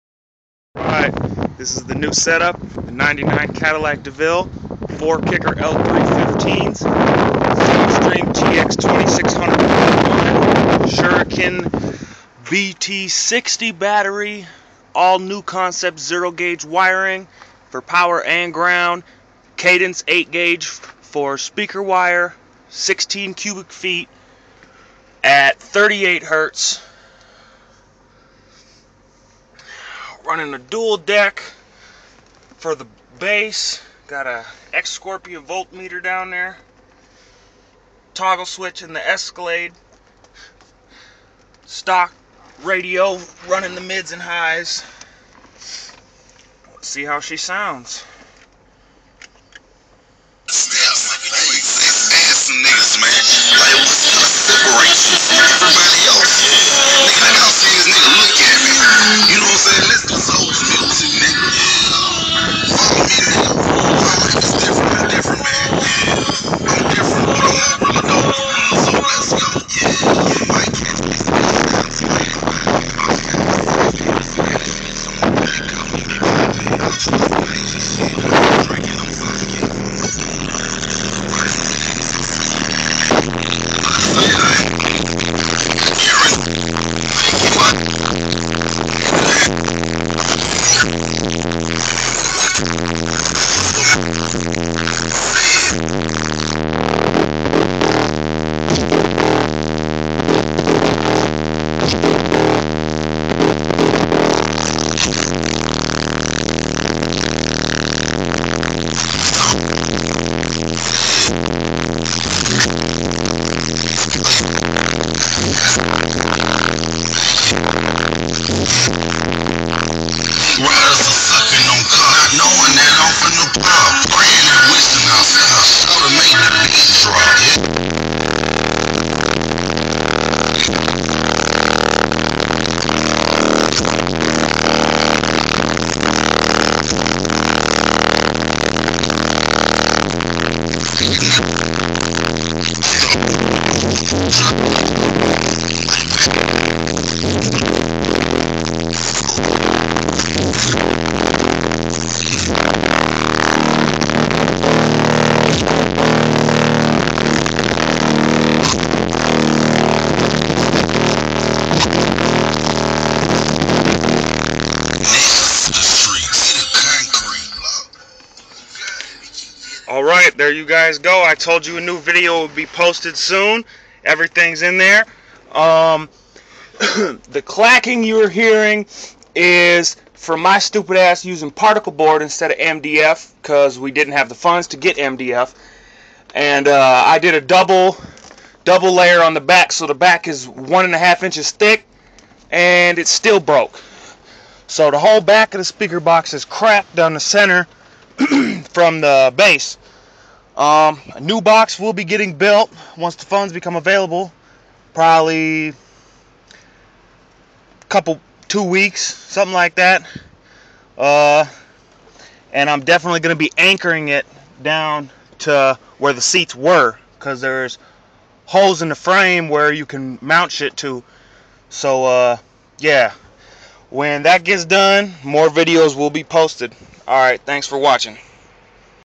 4 kicker l3 15s in a cadillac deville
4 kicker l3 15s...soundstream tx2600.1...shuriken sk-bt 60 batterie...knukonceptz o gaugepower and ground...big 3...cadence 8 gauge for speaker wire...